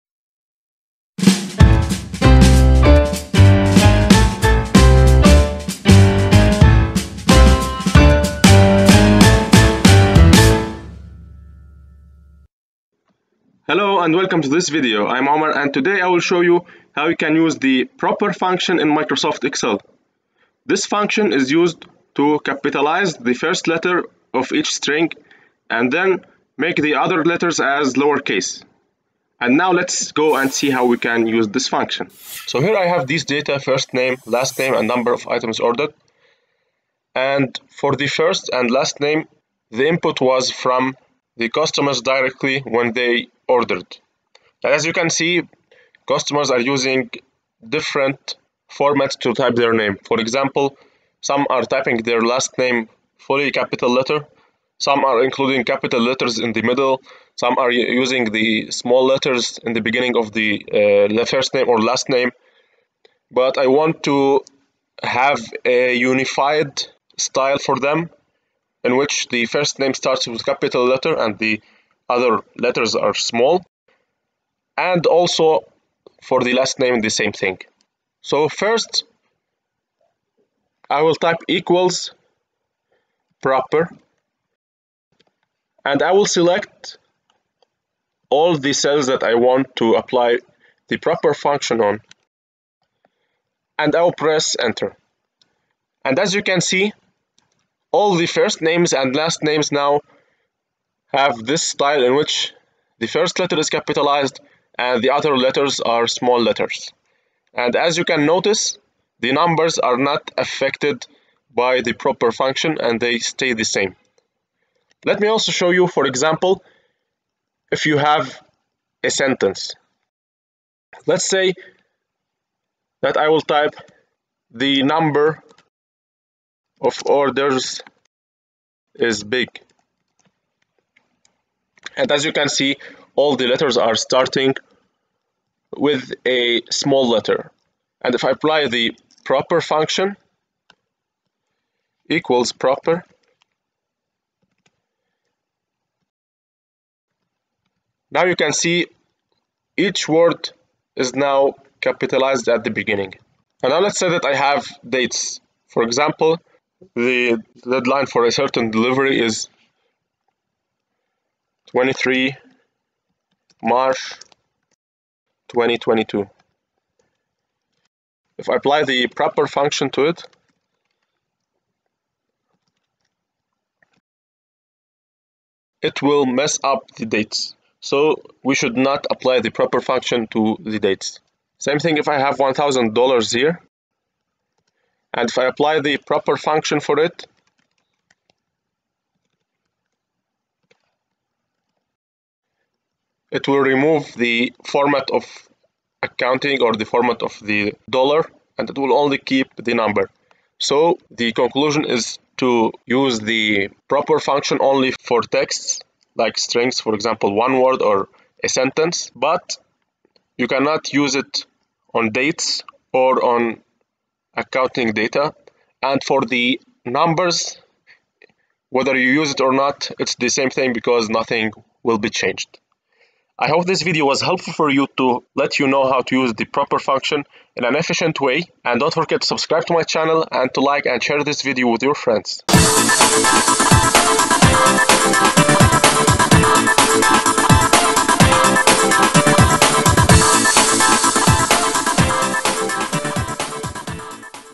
How to use PROPER Function in Excel
In this video you can learn how to use the PROPER Function in Microsoft Excel.
The Excel PROPER function capitalizes each word in a given text string. Numbers, punctuation, and spaces are not affected.